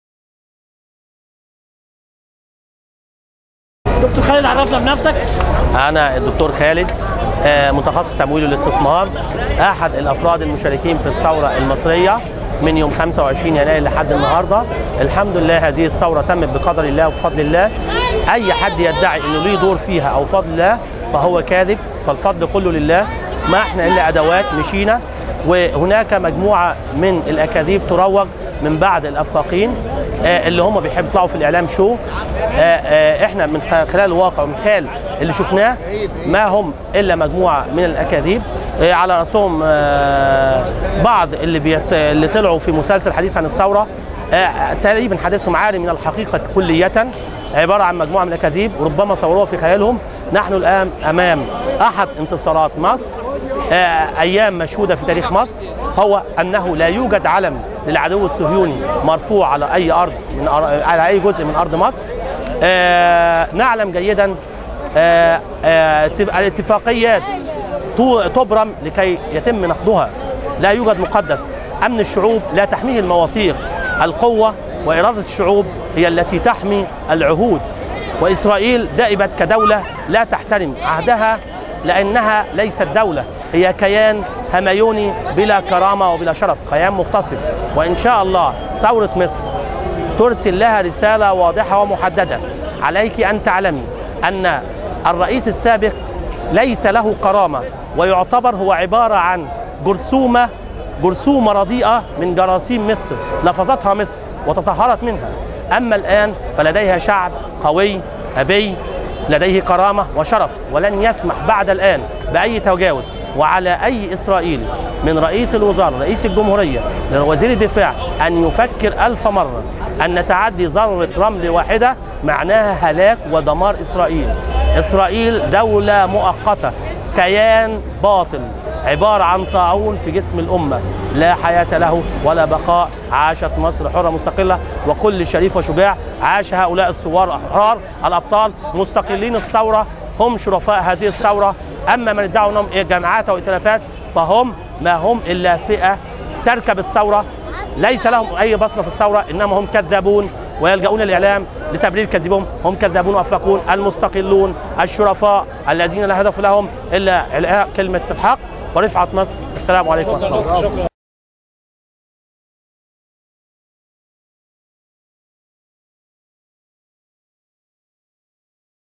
د\خالد يتحدث عن الأكاذيب التى تُرواج بين الثوار
د\خالد يتحدث عن الأكاذيب التى تُرواج بين الثوار من أمام السفارة الإسرائيلية